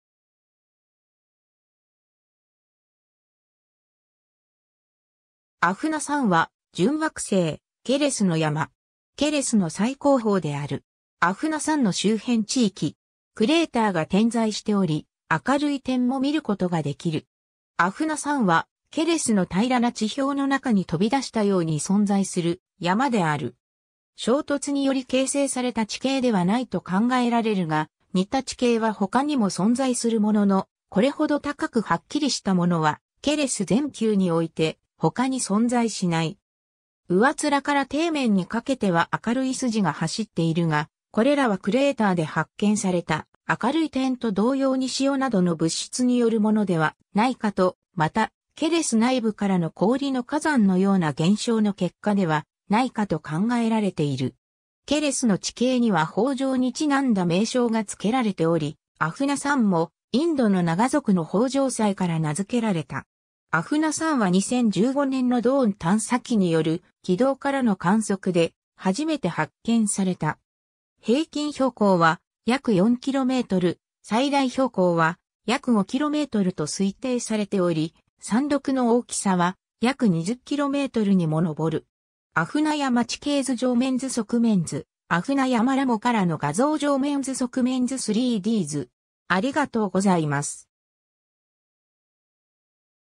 アフナ山
アフナ山（ラテン語: Ahuna Mons）は、準惑星ケレスの山。
ケレスの最高峰である。
 アフナ山の周辺地域。
クレーターが点在しており、明るい点も見ることができる。
 アフナ山は、ケレスの平らな地表の中に飛び出したように存在する山である。
衝突により形成された地形ではないと考えられるが、似た地形は他にも存在するものの、これほど高くはっきりしたものはケレス全球において他に存在しない。
上面から底面にかけては明るい筋が走っているが、これらはクレーターで発見された明るい点と同様に塩などの物質によるものではないかと、またケレス内部からの氷の火山のような現象の結果ではないかと考えられている。
ケレスの地形には豊穣に因んだ名称が付けられており、アフナ山もインドのナガ族の豊穣祭から名付けられた。
 アフナ山は2015年のドーン探査機による軌道からの観測で初めて発見された。
平均標高は約4km、最大標高は約5kmと推定されており、山麓の大きさは約20kmにも上る。
 アフナ山 - 地形図（2016年3月） 上面図 側面図 アフナ山 - LAMO（低高度観測軌道）からの画像（2015年12月） 上面図 側面図 3D図